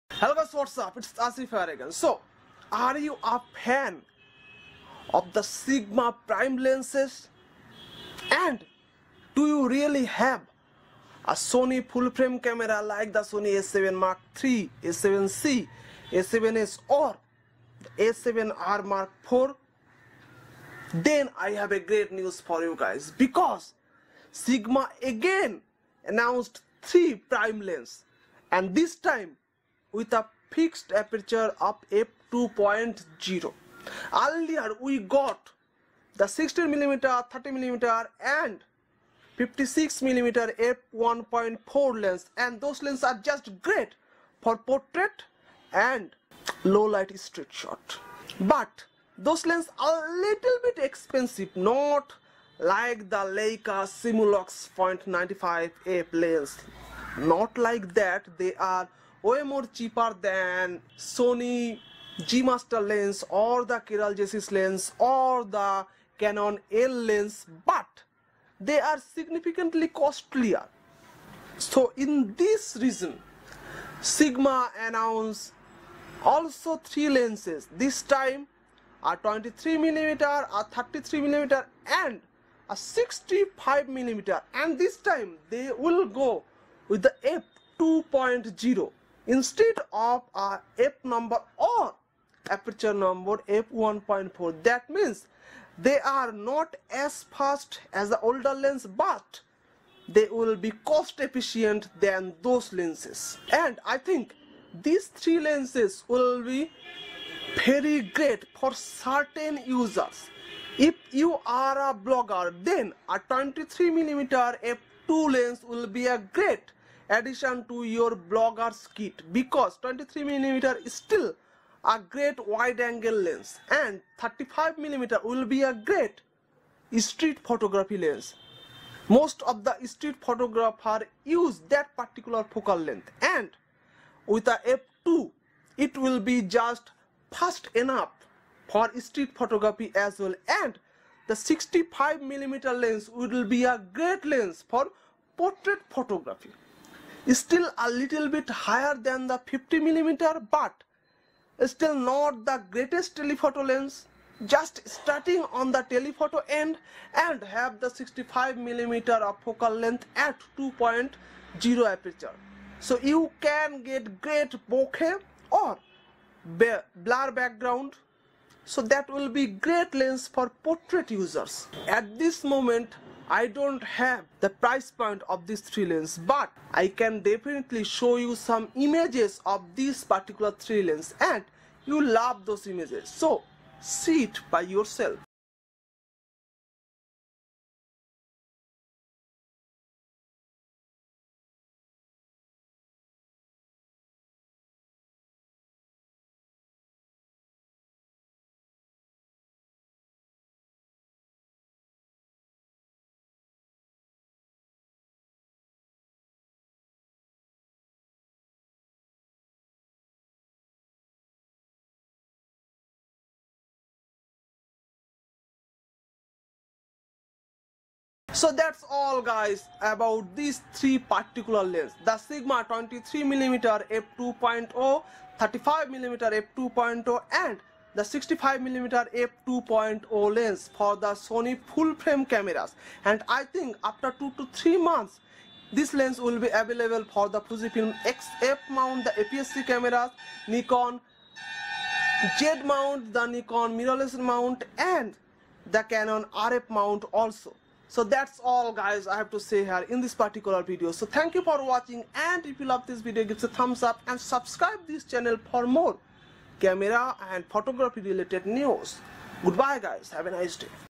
New Sigma 23mm,35mm,65mm f/2.0 lens for Sony E mount and L Mount Cameras (2020)
Sigma just announced the 23/24mm f/2.0 , 35mm f/20. and a 65mm f/2.0 prime lenses.  Previously we have the 16mm f/1.4 , 35mm f/1.4 and the 56mm f/1.4 . They were very fast but also a little bit costly lenses. But these time they reveal the f/2.0 lens which are still very fast but not as costly as the f/1.4's are. So its a great news for the prime lens users. And interestingly they offer 3 different focal range which are suitable for three different kind of photographers or users.
The 23/24mm is just great for wide angle shots and also a great lens for v-logging
The 35mm is just perfect for street photography
and
the 65mm is just great for portrait photography .It's little bit longer than the 50mm but also a little bit shorter than the 85mm . So it's just filling the gaps in those two focal ranges.

Buy the Sigma Lenses :
Sigma 16mm F/1.4
Sigma 35mm F/1.4
Sigma 56mm F/1.4